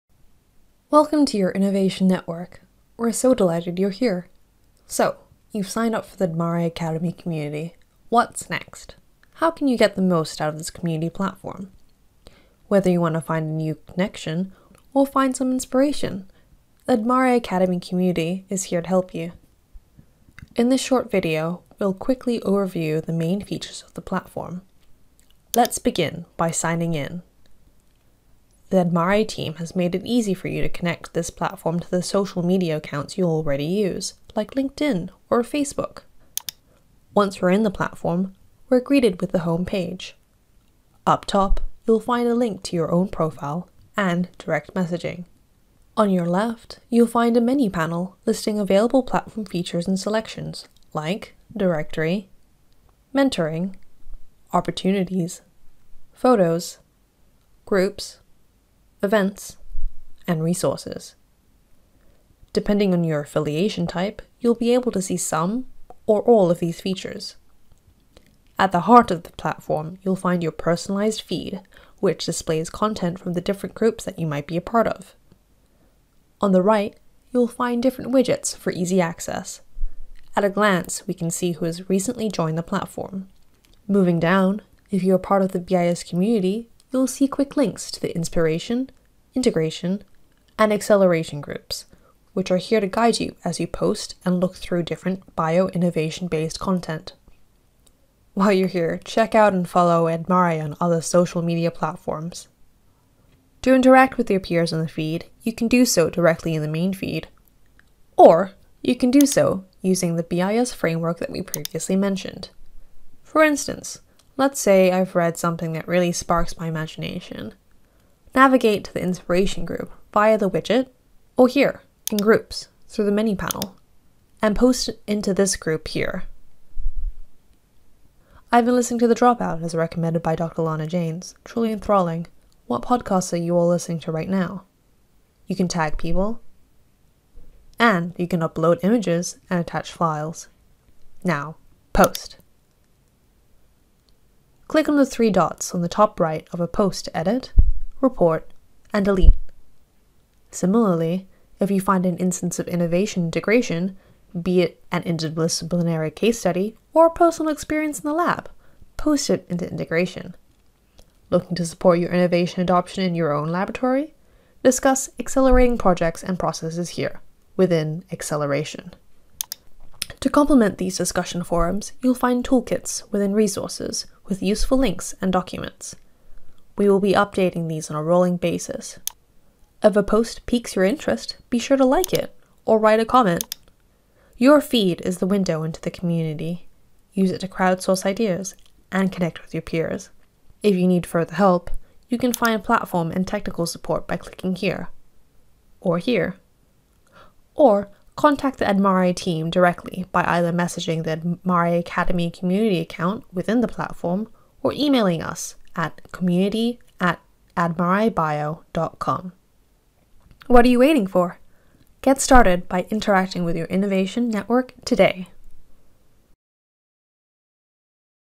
adMare Academy Community - Getting Started Basic Navigation
Find out how to get the most use out of this community and its features by watching the video!